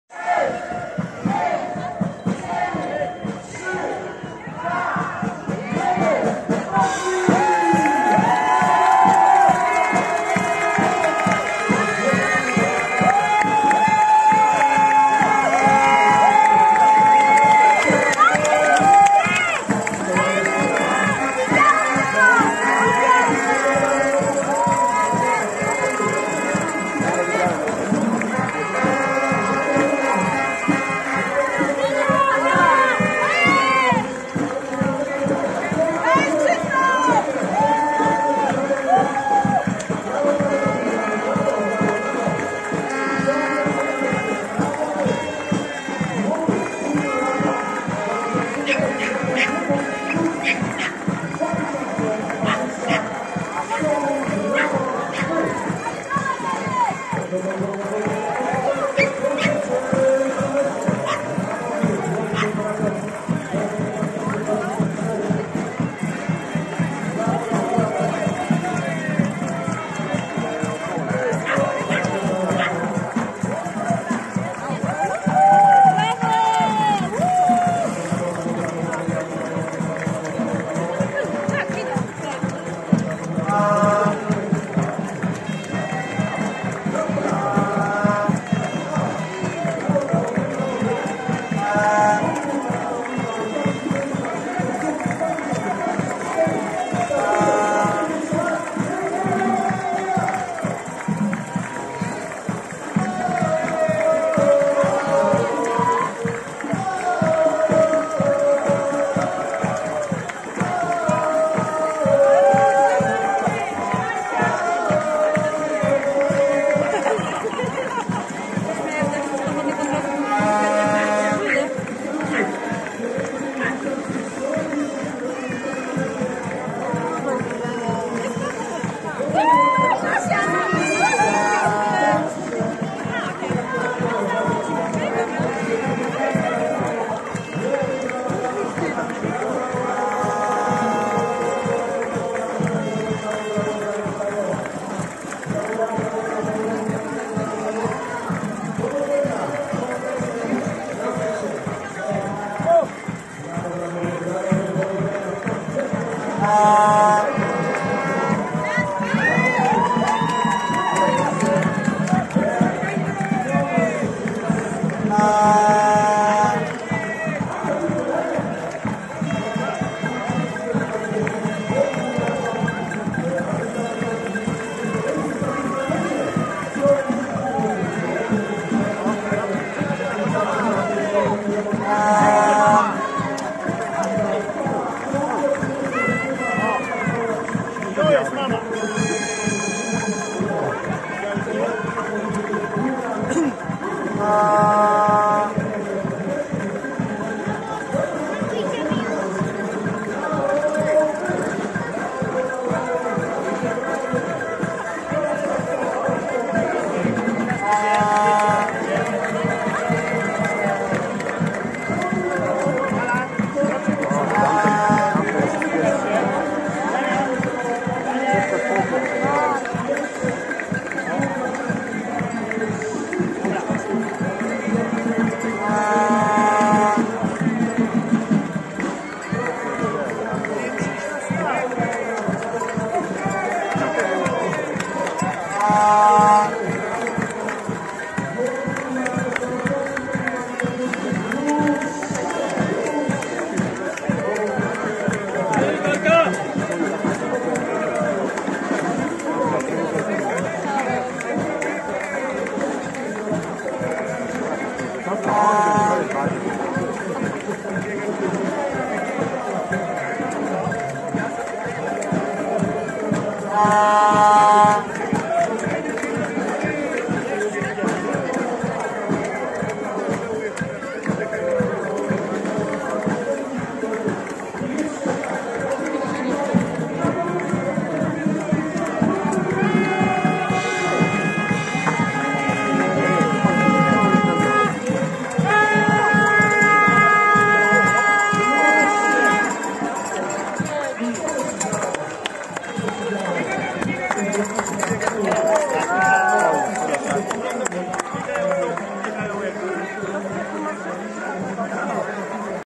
Złoty Półmaraton 21 km - start biegu - Dolnośląski Festiwal Biegów Górskich 2024
Cały start biegu Złoty Półmaraton 21 km na Dolnośląskim Festiwal Biegów Górskich 2024 w Lądku-Zdroju